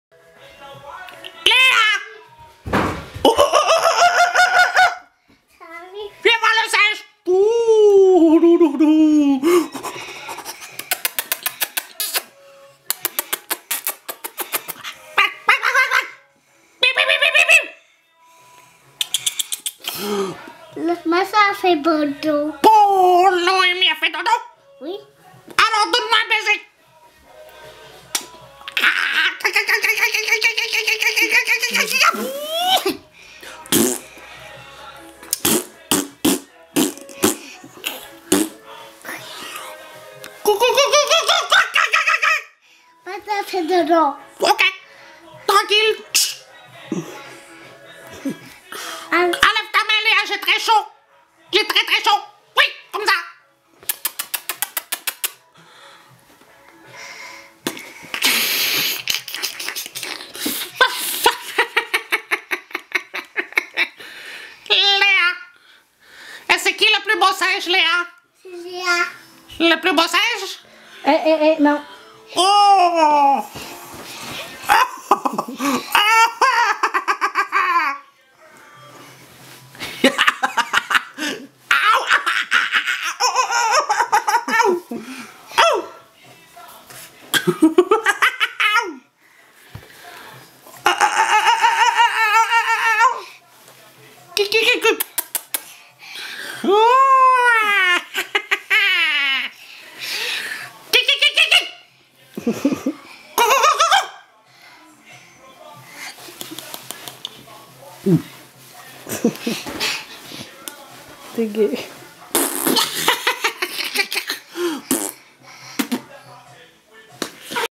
Le Singe!!!
Je n'avais pas le choix de le faire... Il y a quelques mois j'ai trouvé un petit vidéo ou un père de famille faisait la même chose avec ses enfants et j'ai trouvé ca tellement drôle... Je me suis alors dit qu'un jour, j'allais le faire moi aussi... Voilà... C'est ça que ça donne par chez nous...